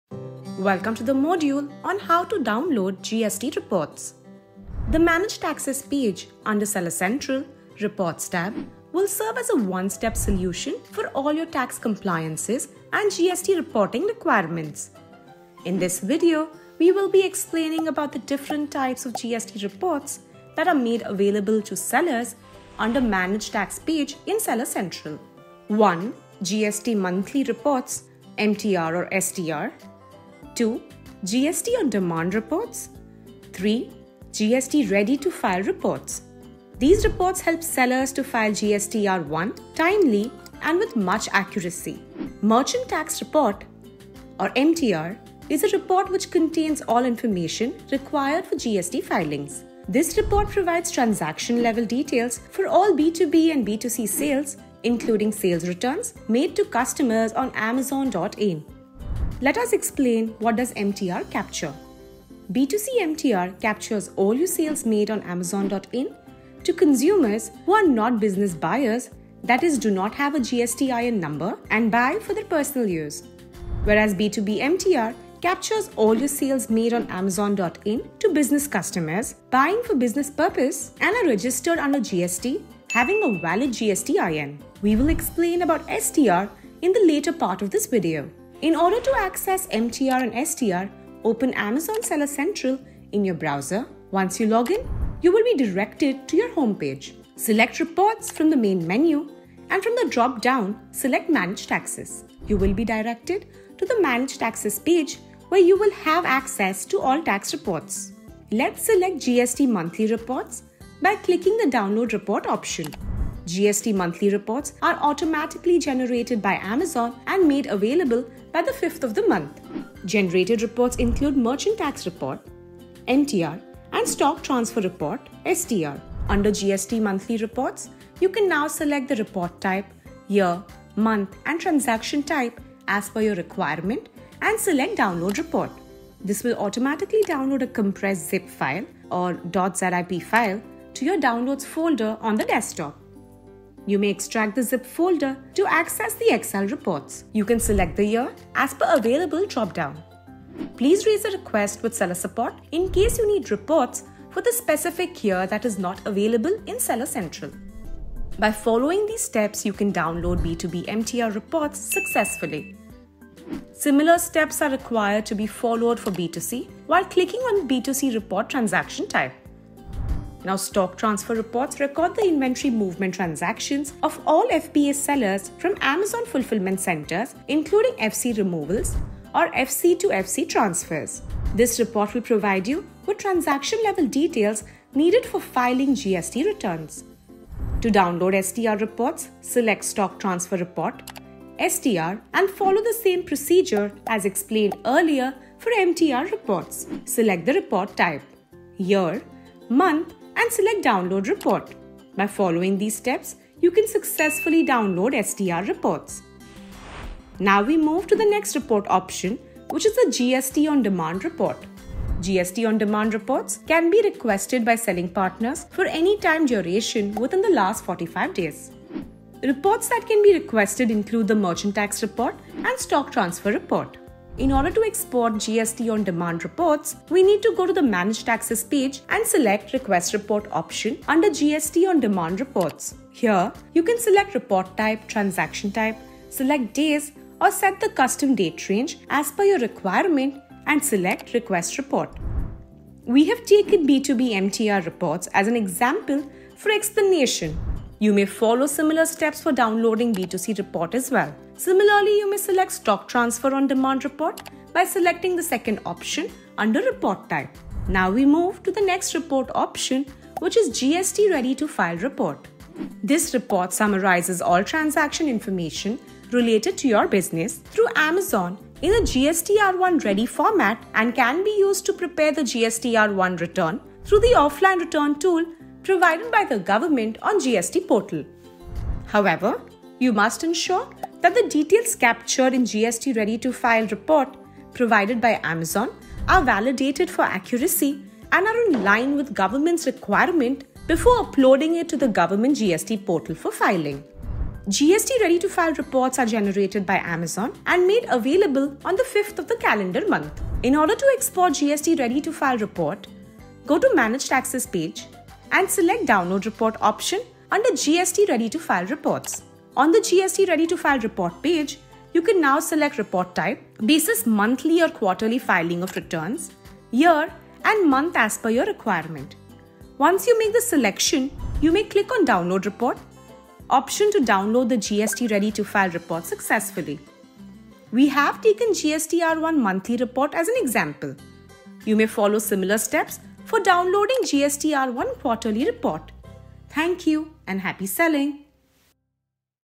How to download GST report | Seller University | Amazon India
In this video, you will learn how to download reports such as MTS, STR, GST on Demand Report, GST Ready to File Report.
0:00 - 0:41 Introduction on Type of Reports
0:42 - 1:28 Explanation on MTR
1:29 - 1:47 How to download MTR/STR from SC
1:48 - 2:49 GST Monthly Reports
2:50 - 3:26 Explanation on STR
3:27 - 4:17 GST on Demand Report
4:18 - 5:39 GST Ready to file Report

Thank You and Happy Selling!

For further learning, visit: "How To Avail GST Registration In a New State"